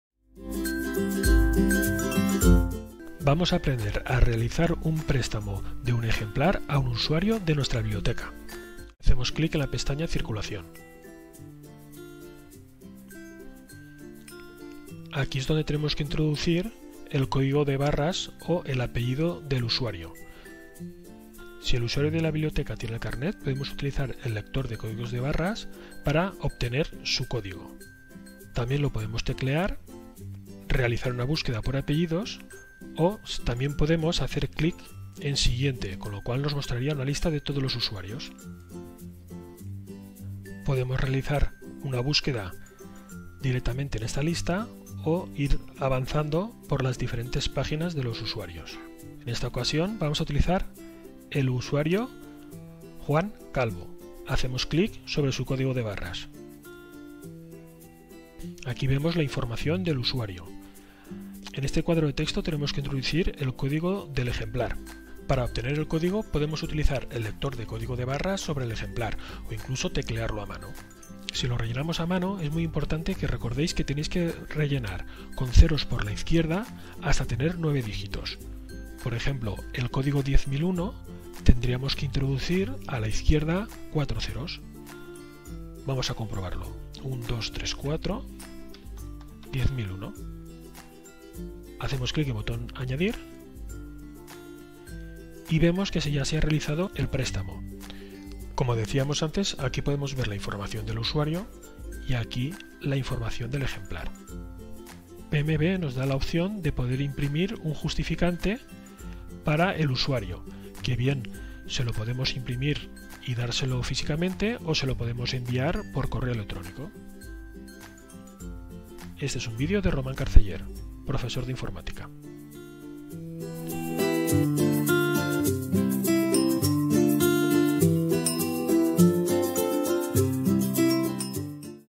PMB en línea - Préstamo
Este vídeo te muestra cómo registrar en PMB un préstamo de un ejemplar a un usuario de la biblioteca de tu centro. Este es un vídeo de apoyo al recurso educativo "Programa de gestión de bibliotecas escolares: PMB" de Cristian García